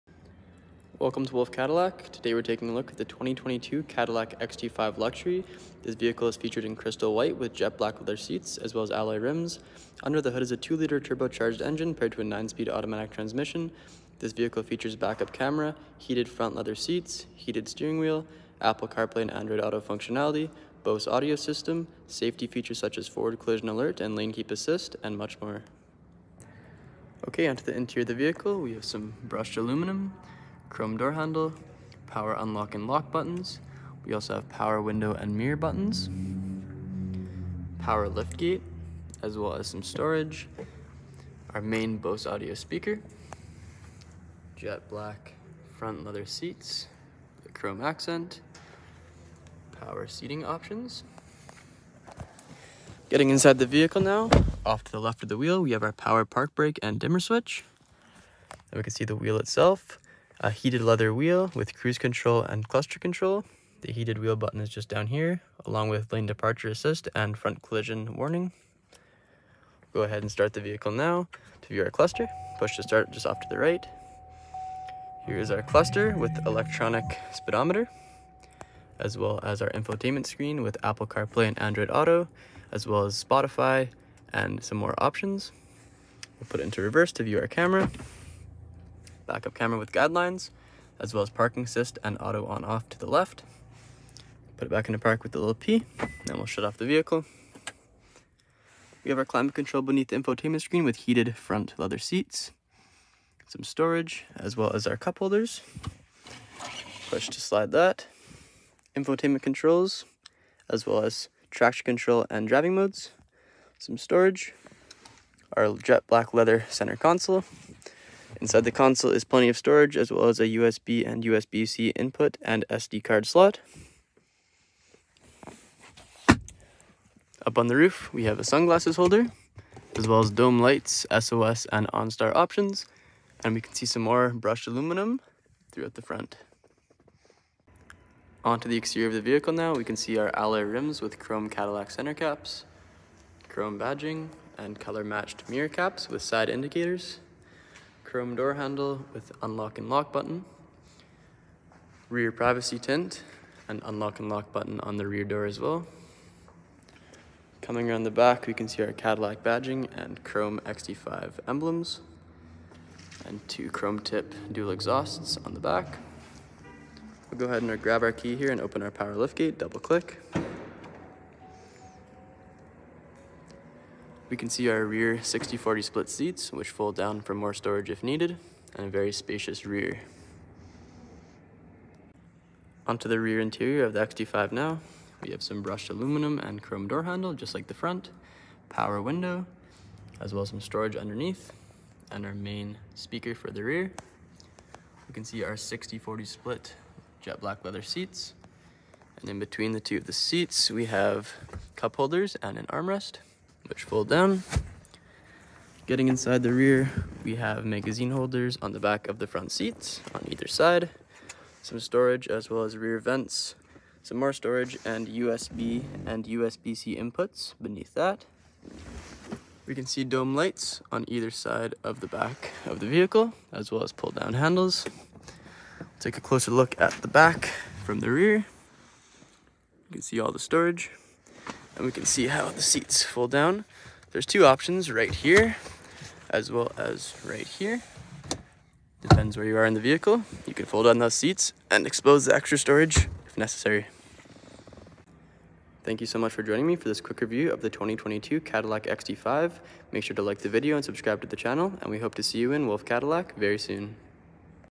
2022 Cadillac XT5 AWD Luxury Review - Wolfe Cadillac Edmonton
If you seek the thrill of driving a sporty and intuitive sport utility, the 2022 Cadillac XT5 Luxury package offers the perfect blend of performance, economy, and luxury. The beautifully designed SUV sports the Cadillac emblem on a slotted grill, beautiful LED waterfall headlamps and sleek body styling. The 18-inch chrome wheels add to the beauty that is XT5. This crossover is powered by a 2.0L 4-cylinder turbo, 9-speed automatic transmission, and is all wheel drive. Exterior features include LED tail lamps, power liftgate, privacy tinted rear windows, bright brushed aluminum roof rails, adaptive remote start and a rear-vision camera with front and rear park assist sensors. The interior cabin seats 5 passengers on premium leather seating and features heated front seats, and dual-zone climate control. The driver features include power windows, power locks, power mirrors, power seating, universal home remote, and a push button engine start control. Safety features include forward collision alert, front pedestrian braking, and Teen Driver technology to keep your young drivers safe. The dash has sleek new design buttons and console shift lever. The media zone has Cadillac styling and XM satellite radio capabilities plus standard Bluetooth phone control, voice activation, Onstar Turn-by-Turn navigation, and smartphone projection. The list of features goes on and on. Stop in to view the XT5 in person or contact our team for more details.

Wolfe Cadillac Edmonton
17820 Stony Plain Rd,
Edmonton, Alberta
T5S 1A4